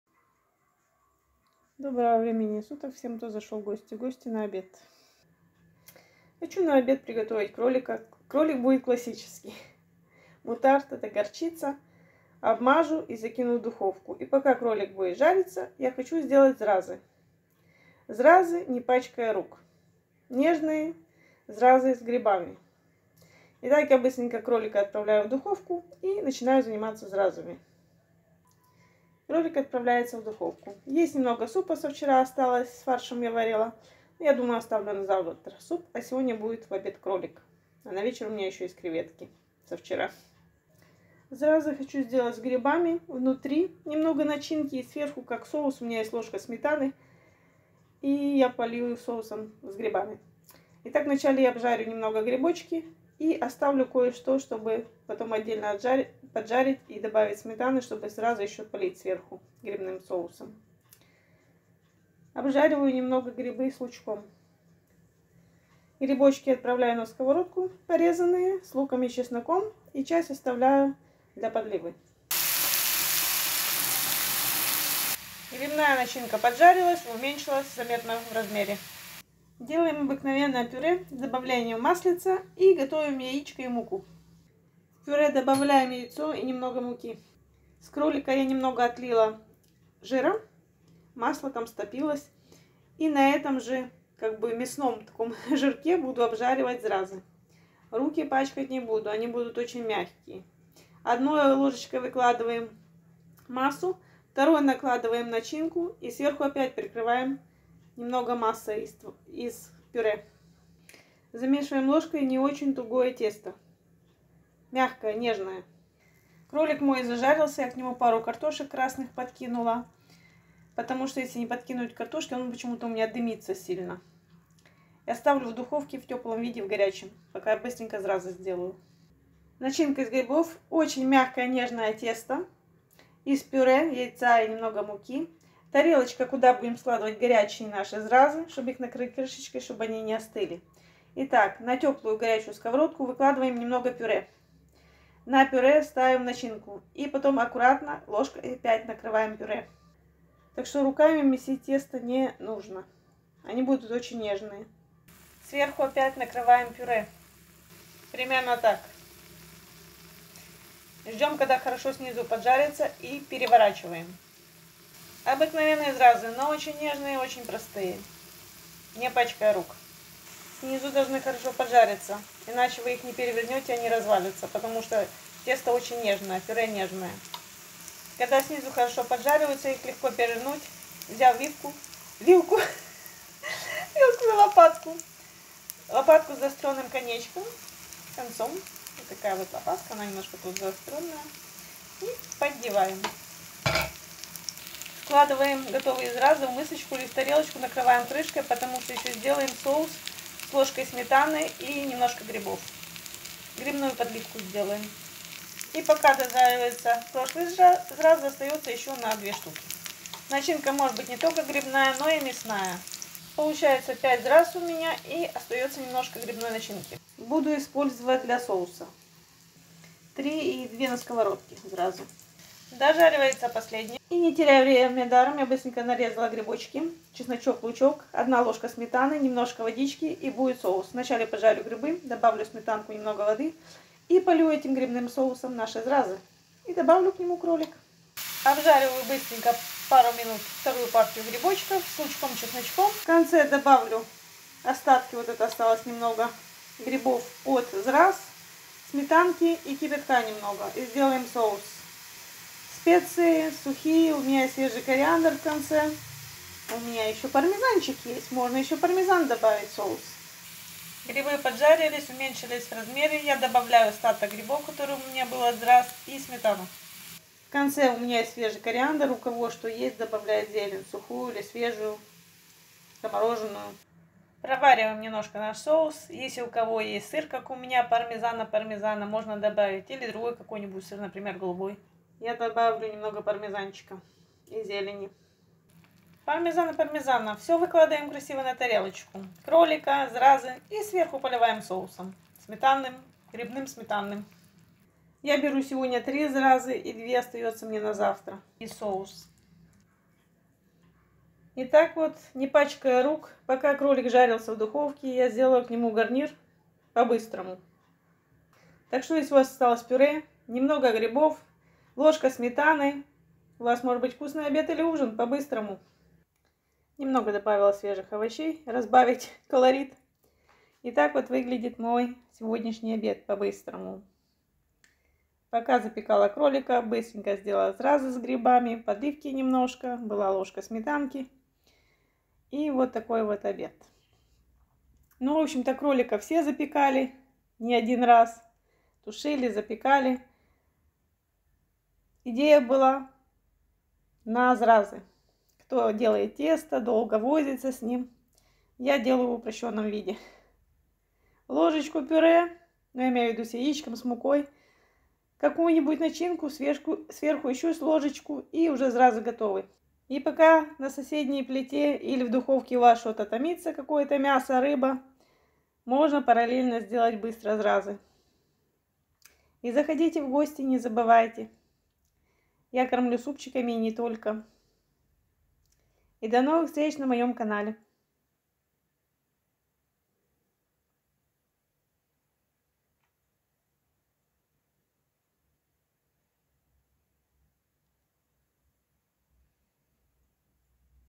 Зразы с грибами 🍄 не пачкая рук и Кролик с грибной подливкой. Париж/Pomme de terre aux champignons.
Классические зразы ускоренная лёгкая версия. К пюре добавить 1 яйцо и пару ложек муки, сделать ложкой мягкое тесто. Поджарить грибы с луком, чесноком. На горячую сковороду ложкой выкладывать картофельную массу, на пюре кладём начинку из грибов, сверху опять прикрыть картошкой. Обжарить аккуратно с обеих сторон. К зразам сделать соус из грибов. Поджарить грибы, добавить сметану и кипяток/бульон, специи, зелень, прокипятить 5-10 минут, если есть можно добавить сыр. Готовые зразы, мясо полить сметанно-грибным соусом.